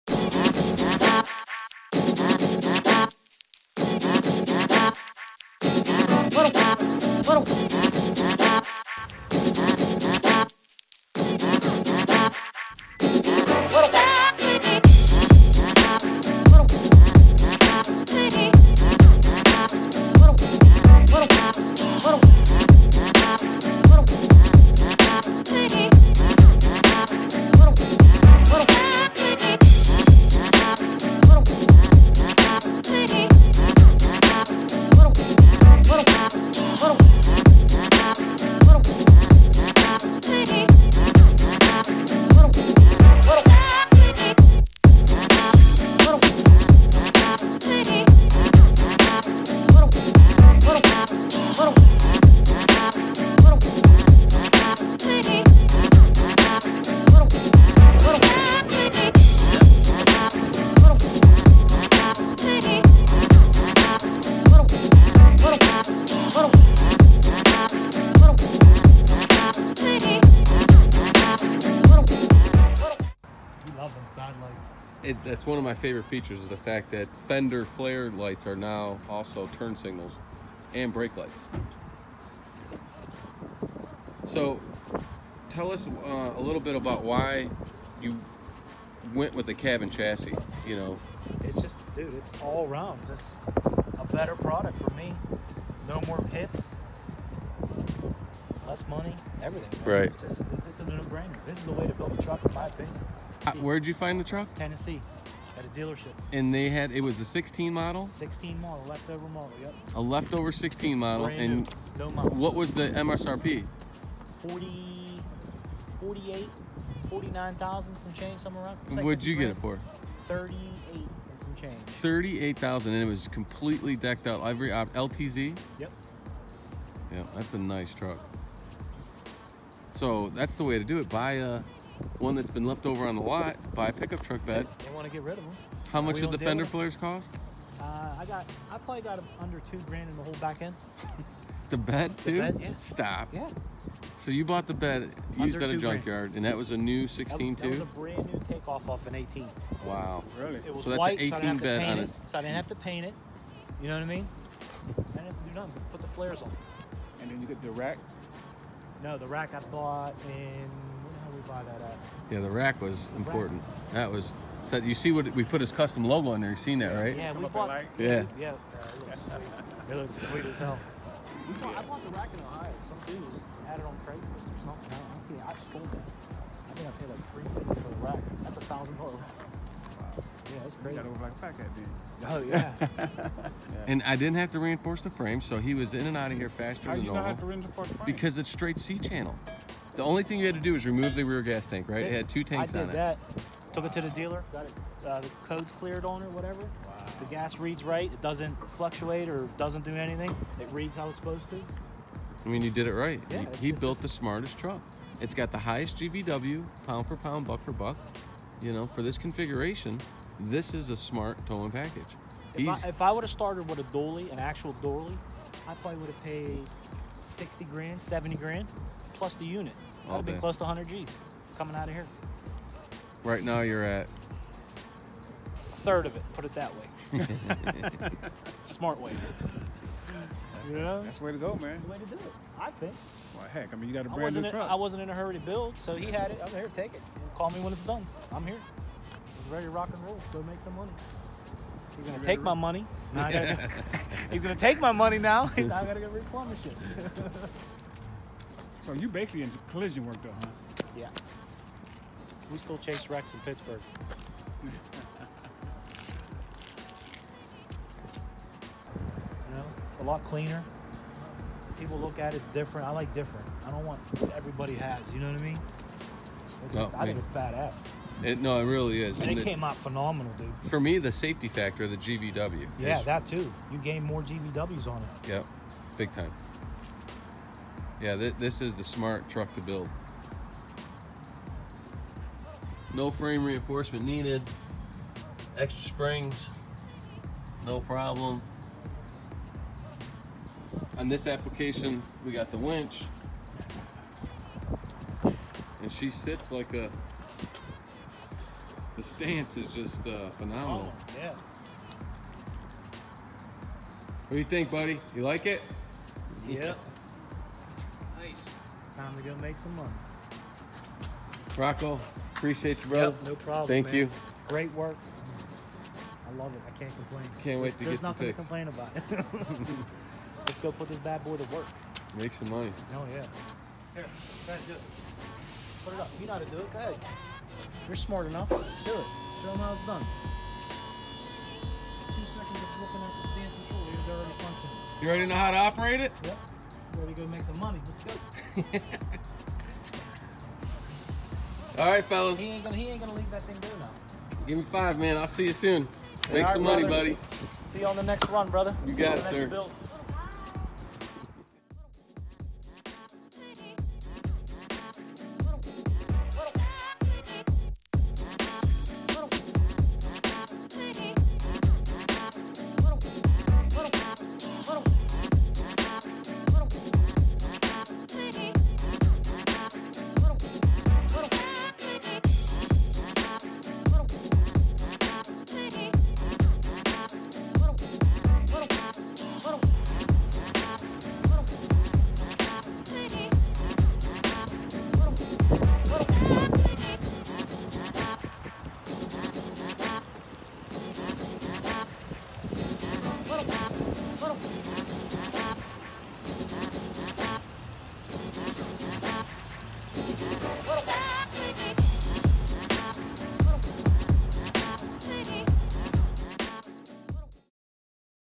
Rocco's Collision Part 2
Here is a perfect example of another successful build for a Repo Wheel Lift or Repo Truck. This one was for Roccos Collision of Pittsburgh. They are happy with their repo truck repo wheel lift & you can also receive the same customer service and overall satisfaction of purchasing a DetroitWrecker product. Made In Detroit. Thank you goes out to Roccos. They are a family business that was a pleasure to work for. They came to DetroitWrecker & you should too!